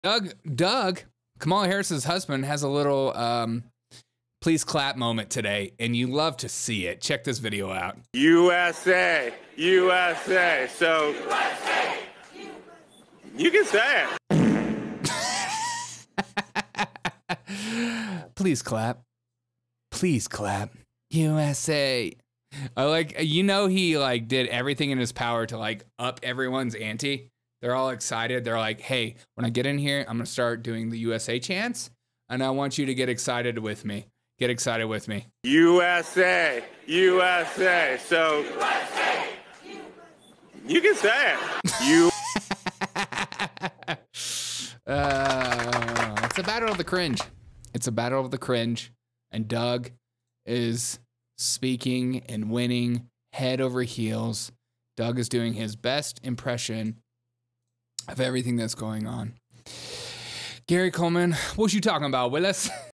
WOW! POLITICIANS husband gets CRINGE!
Get ready for some serious secondhand embarrassment! Kamala Harris’s campaign just delivered one of the most cringe-worthy moments yet, and it’s going viral for all the wrong reasons. From awkward interactions to uncomfortable speeches, this video captures it all. Don’t miss out on the latest campaign mishap that everyone’s talking about!